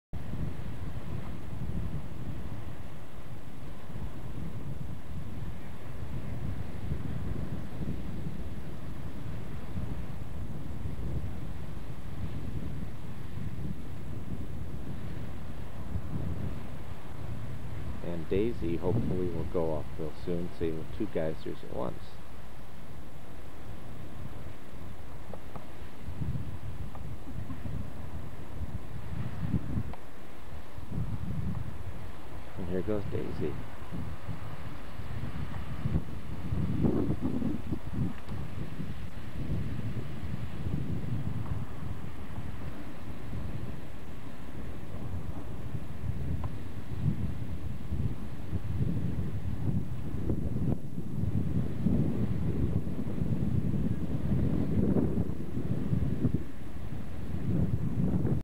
Splendid and Daisy Geysers 7-9-97
Eruption of Splendid Geyser, followed by Daisy Geyser for a dual eruption